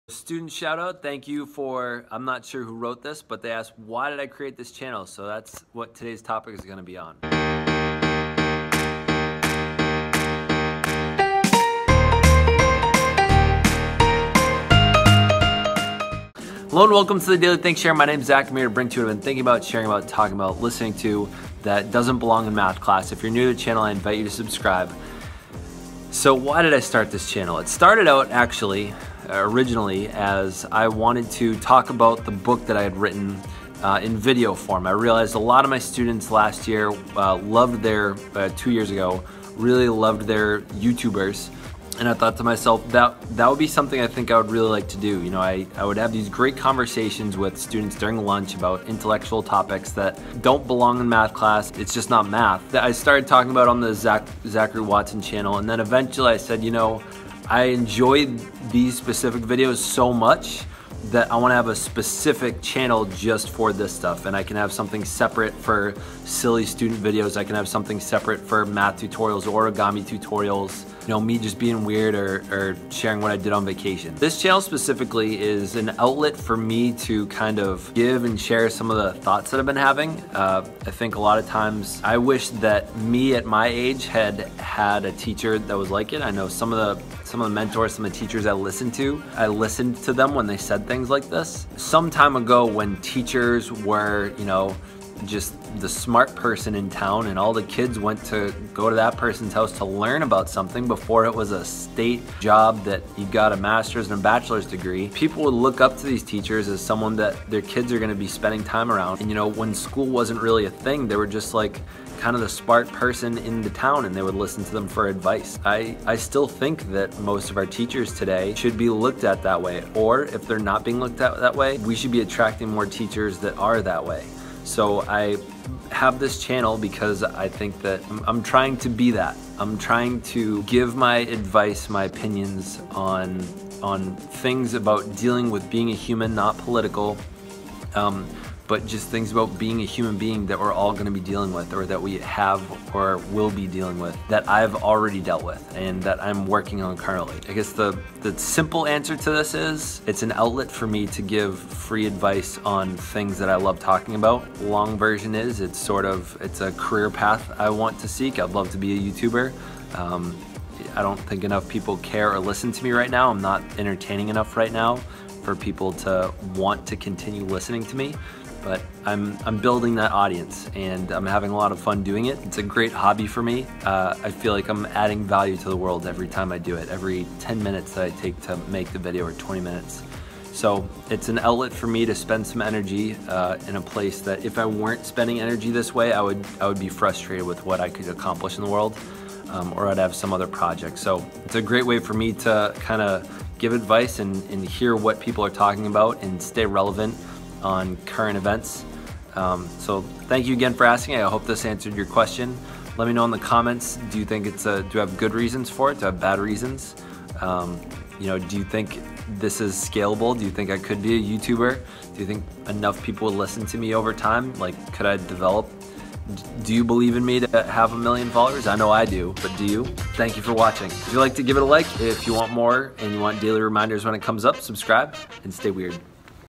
TS 139: Student Question: "Why do You Have This Channel?"
This video is about why i do this channel